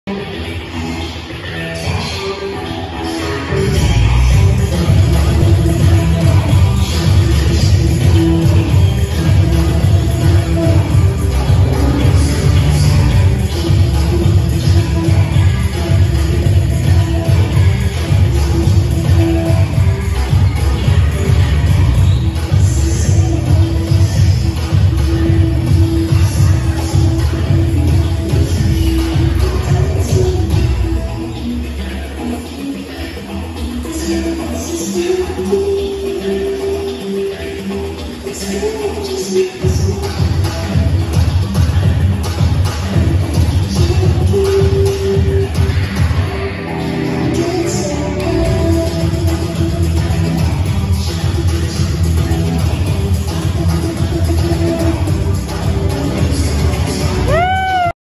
Jazz funk adult dance with Jenny De Tore at Powerhouse Dance South Jersey
Powerhouse Dance offers live and virtual dance classes for adults. Based in Cherry Hull, New Jersey.